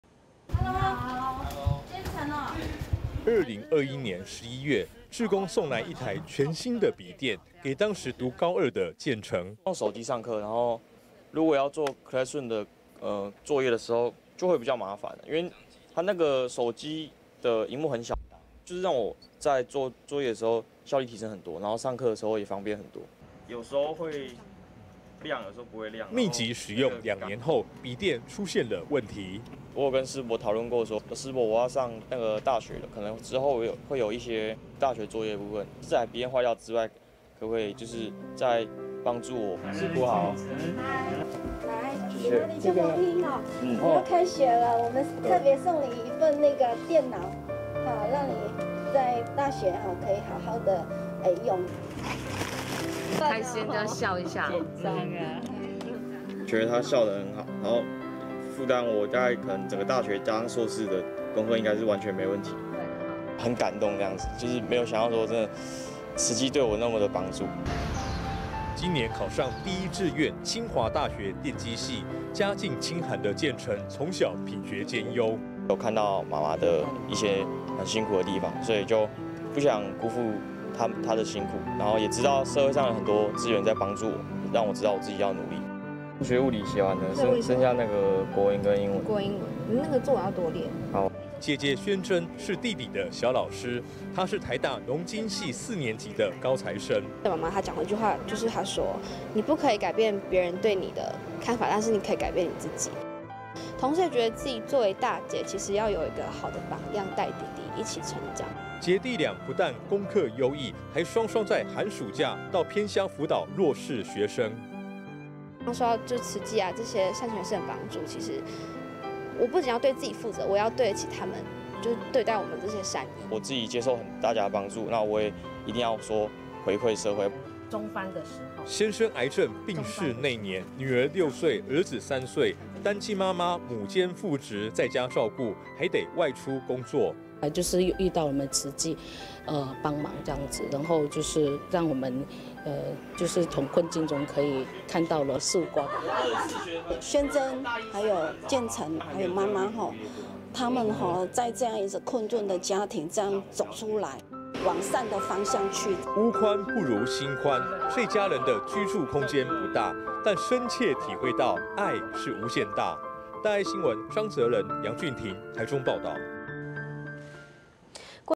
有愛付出就富足 新芽助學生回饋社會
新芽獎系列報導，今天開始一連五天，都要帶給您這些孩子的勵志故事，今天故事主角是一對姊弟，小時候爸爸因為癌症過世，全靠媽媽一人撐起家庭，因為經濟困苦，他們沒辦法像其他同學一樣、去補習班，不過他們是加倍努力，分別考上台大和清大。現在的他們、不只成績好，也有感於、成長過程中有慈濟的關懷協助，決定到偏鄉、輔導弱勢，用公益行動、回饋社會。

2021年11月，志工送來一台全新的筆電、給當時讀高二的健誠。

慈濟新芽獎得主 鐘健誠：「(先前)用手機上課，如果要做Classroom的作業的時候，就會比較麻煩，因為手機的螢幕很小，(新筆電)就是讓我做作業的時候，效力提升很多 然後上課的時候，也方便很多。」

密集使用2年後，筆電出現了問題。

慈濟新芽獎得主 鐘健誠：「我有跟(慈濟)師伯討論過說，師伯 我要上大學了，可能之後會有一些，大學作業的部分，這台筆電壞掉之外，可不可以再幫助我。」

鐘健誠 與 慈濟志工 ：「(師伯好) 很高興吧 你要開學了，我們特別送你一份電腦，讓你在大學可以好好地用。」

慈濟新芽獎得主 鐘健誠：「覺得它(這台筆電)效能很好，然後負擔我大概可能整個大學，加上碩士的功課，應該是完全沒問題 很感動這樣子，沒有想到說真的慈濟，對我那麼地幫助。」

今年考上第一志願「清華大學電機系」，家境清寒的健誠，從小品學兼優。

慈濟新芽獎得主 鐘健誠：「看到媽媽一些很辛苦的地方，所以就不想辜負她的辛苦，然後也知道社會上，很多資源在幫助我，讓我知道我自己要努力。」

弟弟 鐘健誠 與 姊姊 鐘宣臻 ：「(數學 物理寫完了)，(剩下國文跟英文)，國文跟英文 你作文要多練 (好)。」

姊姊宣臻、是弟弟的小老師，她是台大農經系四年級的高材生。

慈濟新芽獎得主 鐘宣臻：「媽媽她講過一句話說，就是她說 你不可以改變，別人對你的看法，但是你可以改變你自己，同時覺得自己作為大姊，其實要有一個好的榜樣，帶弟弟一起成長。」

姊弟倆不但功課優異，還雙雙在寒暑假，到偏鄉輔導弱勢學生。

慈濟新芽獎得主 鐘宣臻：「受到慈濟這些善心人士的幫助，其實我不僅要對自己負責，我也要對得起他們，對待我們這些善意。」

慈濟新芽獎得主  鐘健誠：「我自己接受大家的幫助，我也一定要說回饋社會。」

先生癌症病逝那年，女兒6歲、兒子3歲，單親媽媽、母兼父職在家照顧，還得外出工作。

鐘健誠的媽媽 符文君：「就是遇到我們慈濟幫忙，然後就是讓我們從困境中，可以看到了曙光。」

慈濟志工 陳麗花：「宣臻 還有健誠 還有媽媽，他們在這樣一個困頓的家庭，這樣走出來 往善的方向去。」

屋寬不如心寬，這家人的居住空間不大，但深切體會到「愛是無限大」。

張澤人 楊俊亭   台中報導